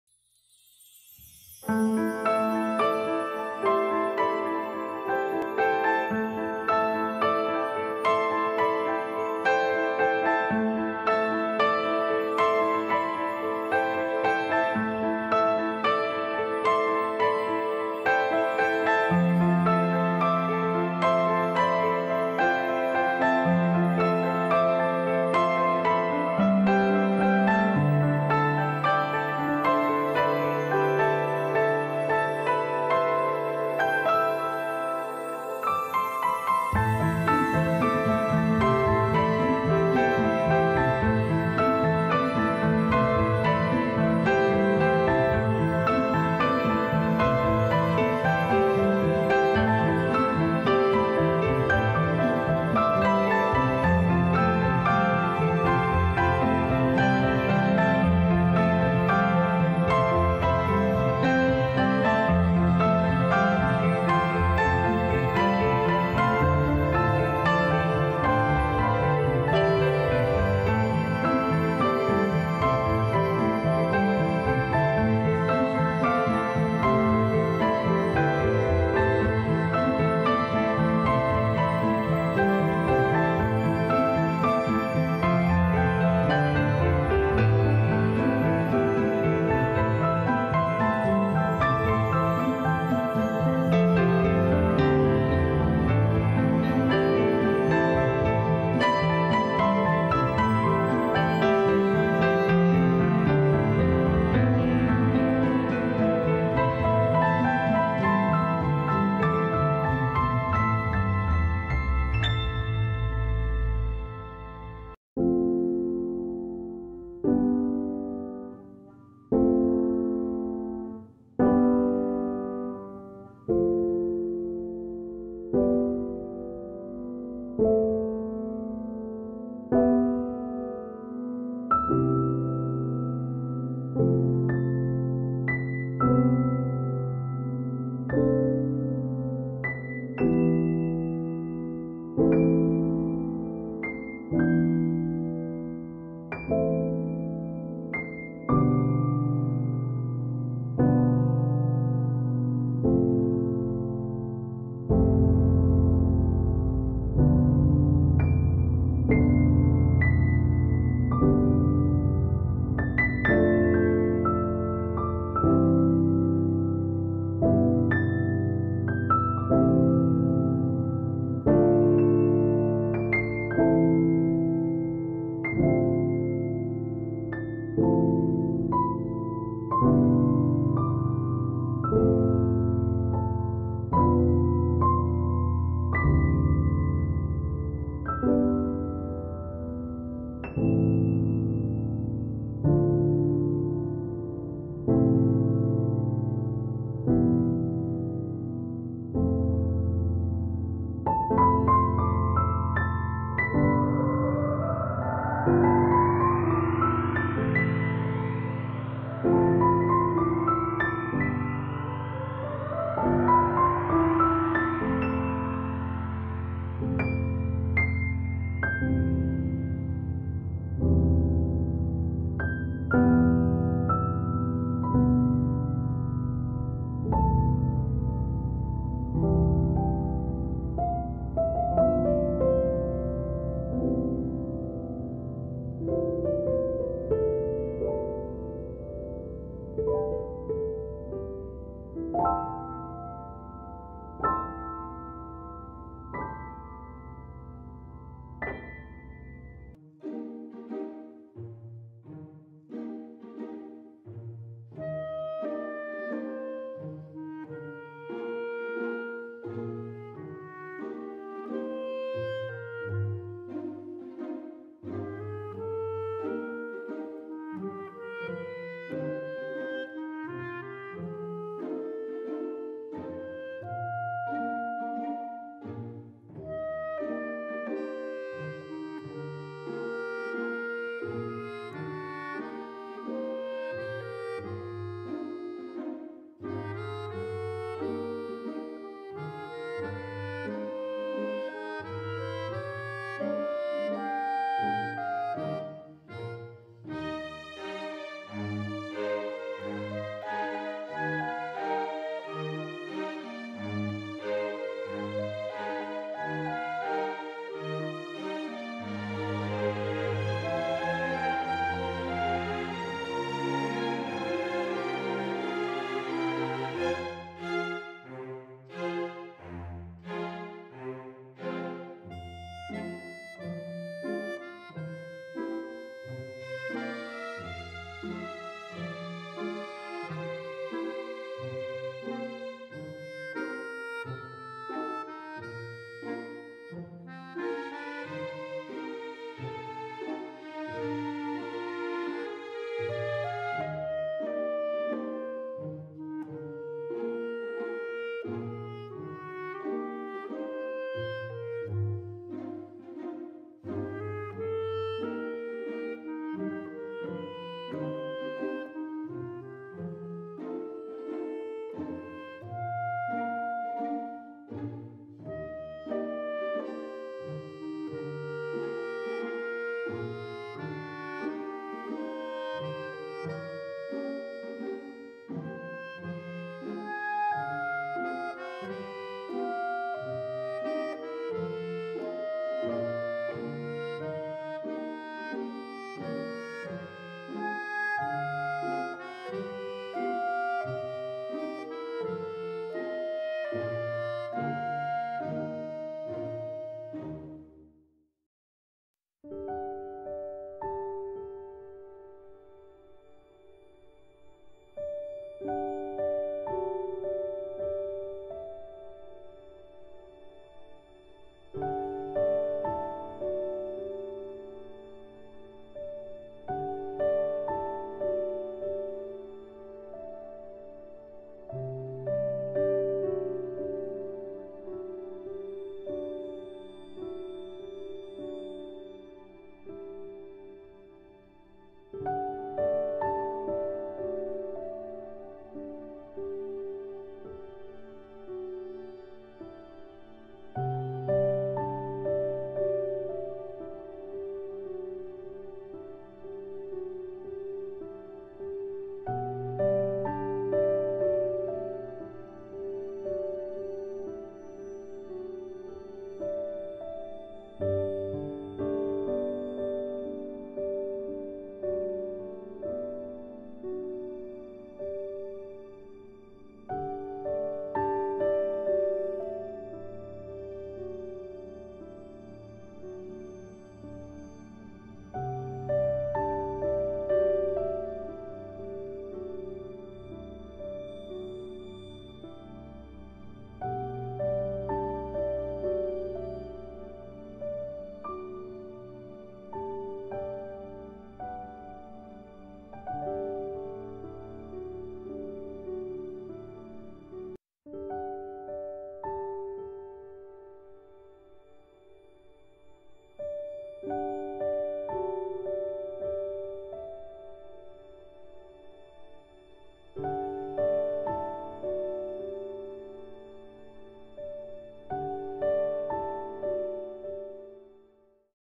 Christmas Celebration In Cochin Churches In Covid19 Situation | ഈ കോവിഡിൽ കൊച്ചിയിലെ പള്ളികളിലെ Xmas
In this video on "Just Vlogging All" shows how the Churches in Cochin (Kochi/കൊച്ചി) in Kerala celebrated Christmas during the Corona / Covid19 spread in the year 2020.
Just Vlogging All wishes Christmas greetings to all it's viewers.
Merry Christmas.